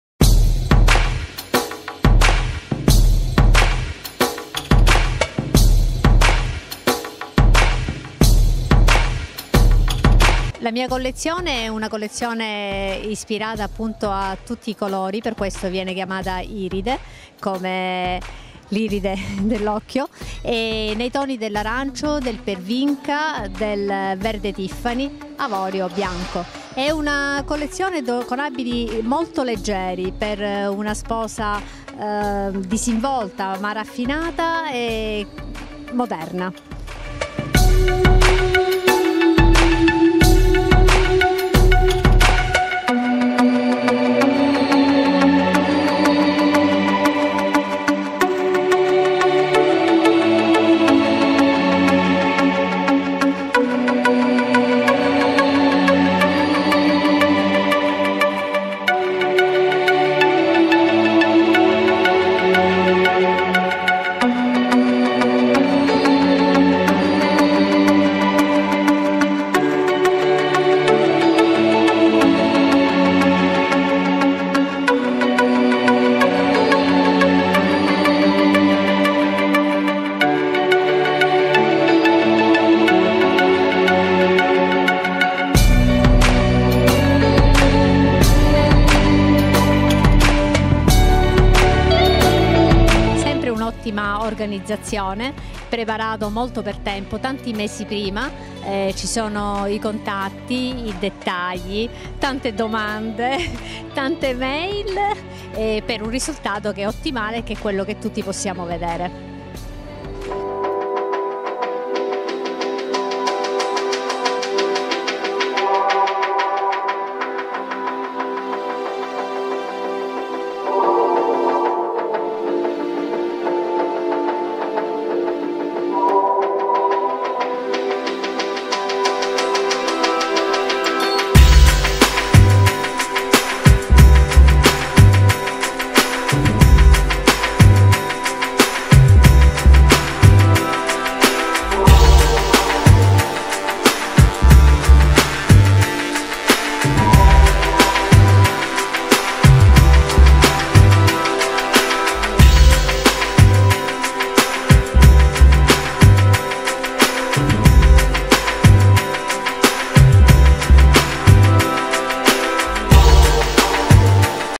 KASTA MORRELY FASHION WEEK S/S 2016 Interview Tina Arena
KASTA MORRELY FASHION WEEK S/S 2016
Interview with de italian designer Tina Arena
TINARENA